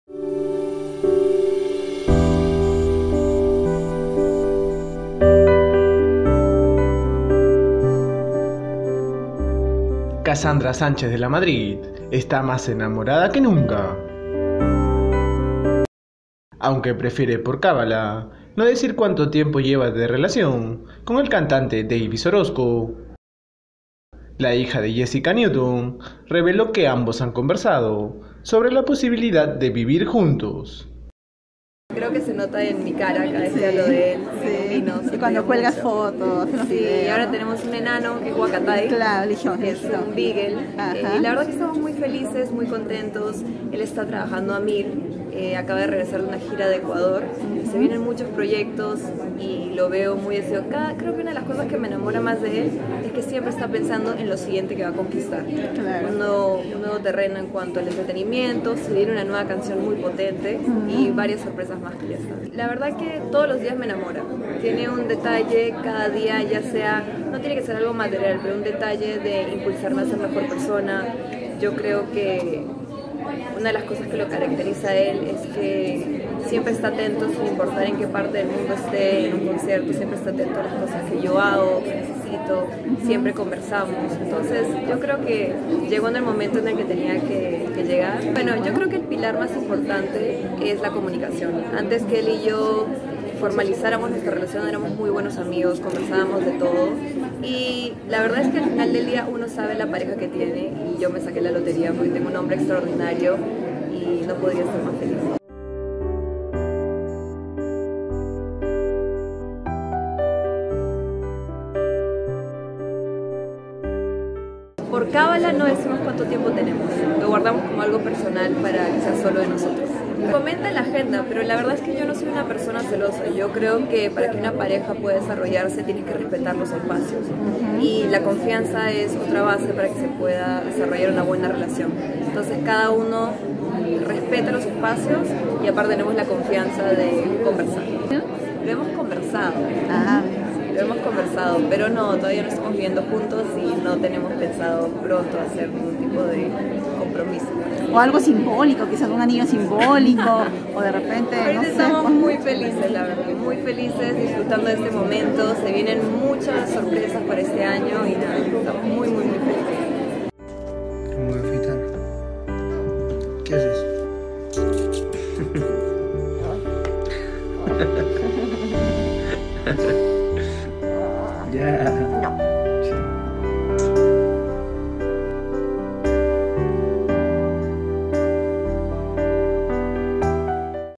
Cassandra Sánchez habla sobre planes de convivencia con Deyvis Orosco: “Me saqué la lotería con él”
¡Que viva el amor! Cassandra Sánchez nos cuenta cómo va su relación con Deyvis Orosco y habla sobre planes de convivencia: “Me saqué la lotería con él”